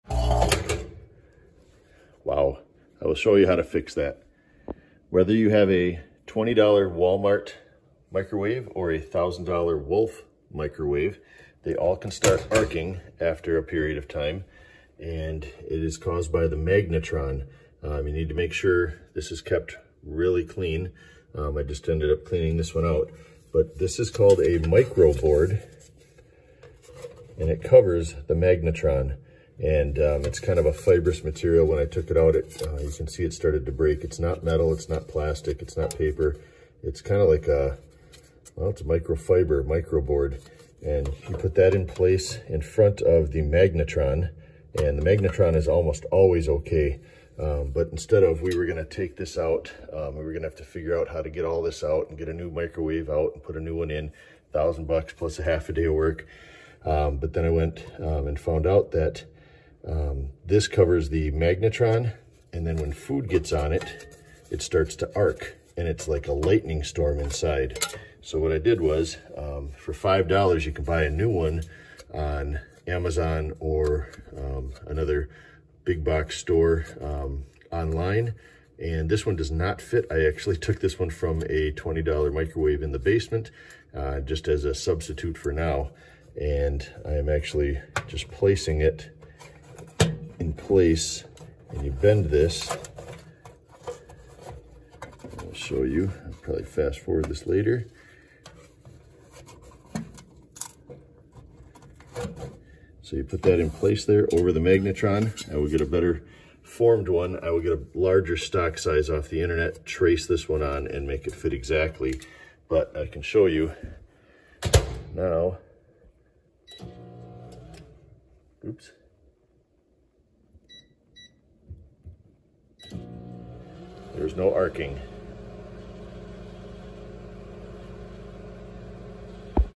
Why is my microwave sparking and arcing? Fix it for $5.
Arcing and sparking microwave.   Don't throw it away.  Fix it for 5$